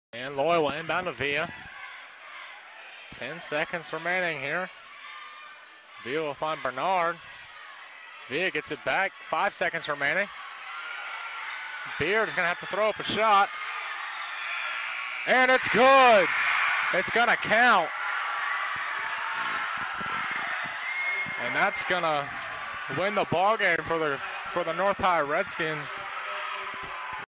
Wichita North #1 Zach Beard wins game off last second jumper
Wichita North and Wichita Northwest square off in a Greater Wichita District game.